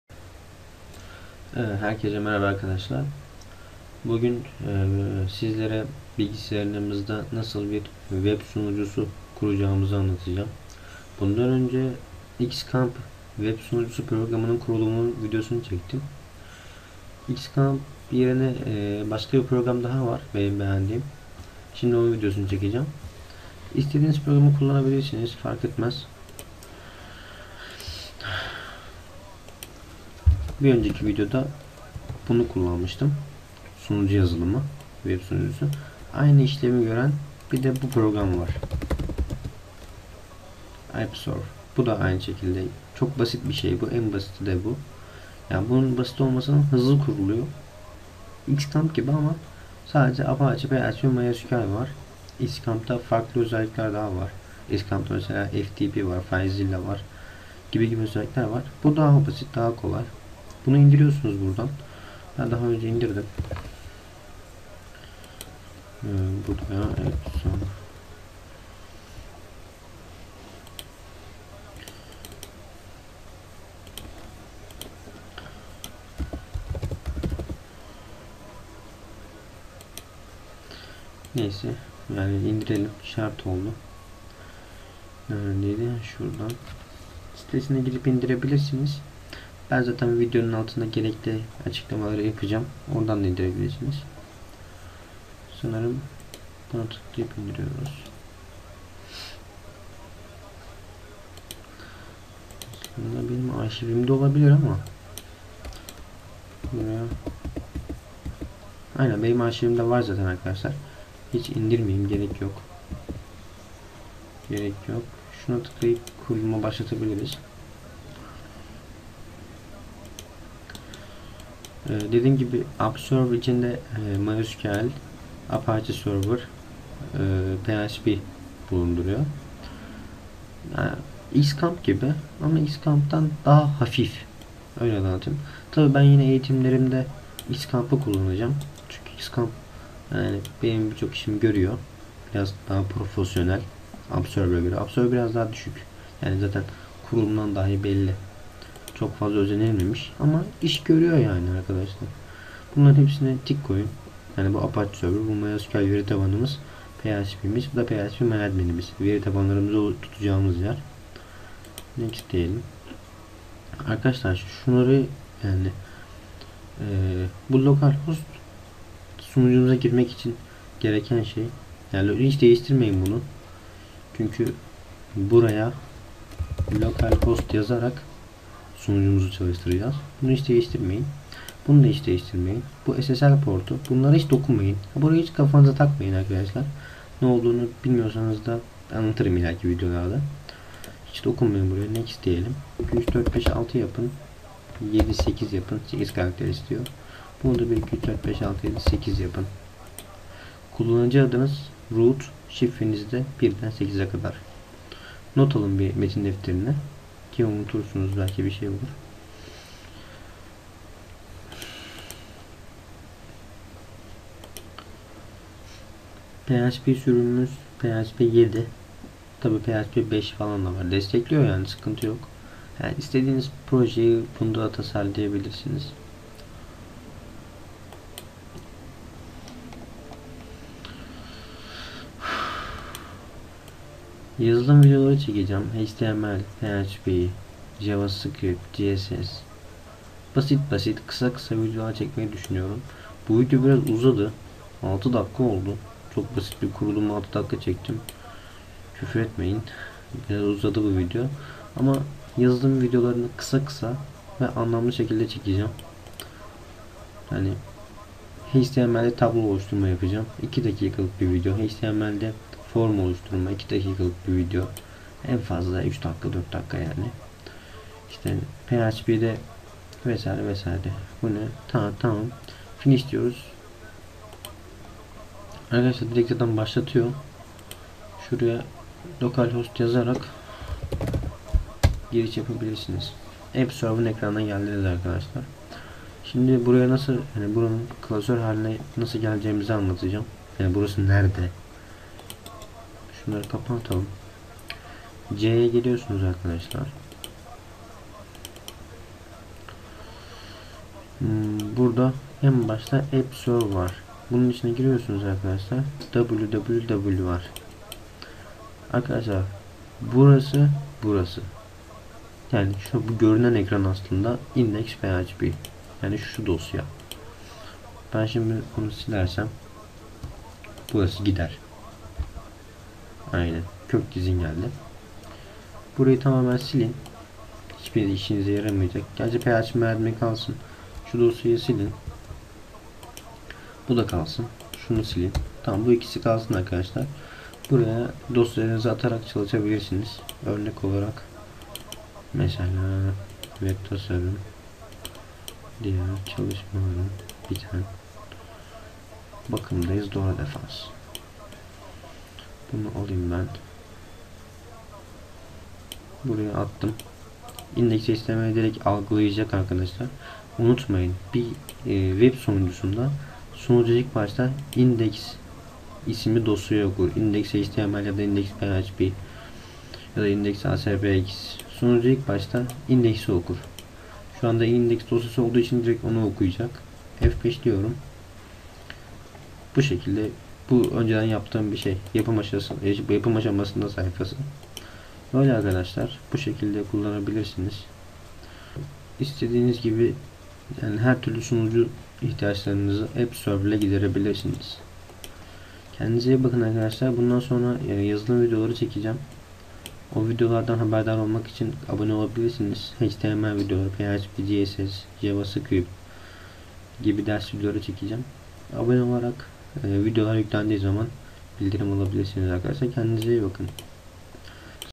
AppServ Nedir Kurulumu
Appserv Tıpkı Xampp gibi bilgisayar üzerinde PHP+MYSQL+HTML vs dosyaların çalışması için web sunucu hizmeti veriyor. Appserv kurarak bilgisyarınızda hızlıca bir web server kurabilirsiniz.